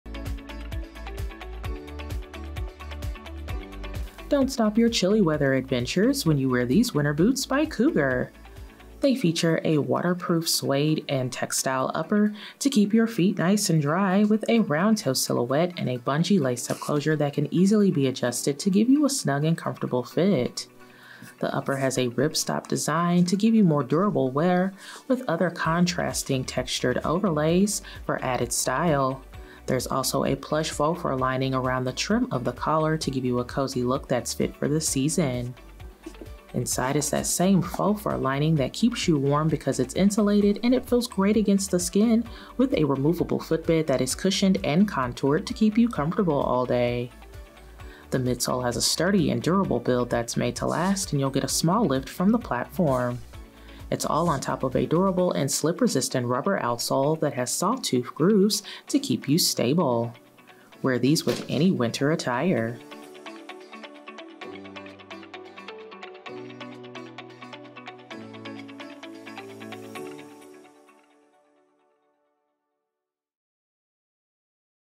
Cougar Vibe Waterproof SKU: 9811366
Your chilly weather adventures will not be stopped due to any obstacle when the  Cougar® Vibe Waterproof boots have got your back.
Nylon and suede upper.
Polar plush lining.
Removable molded foam footbed.
Always Waterproof technology offers seam-sealed construction and thermoplastic polyurethane membrane under the lining as an added barrier.
Temperature rated -24ºC/-11ºF.
Faux fur-lined collar ensures added warmth.
Front-toggle closure.
Round toe silhouette.
Polyurethane midsole.
Slip-resistant TPR outsole.
Imported.
Measurements:Heel Height: 1.25 in;Boot Shaft Height: 5.25 in;Shaft Circumference: 10.75 in;Weight: 19.05 oz.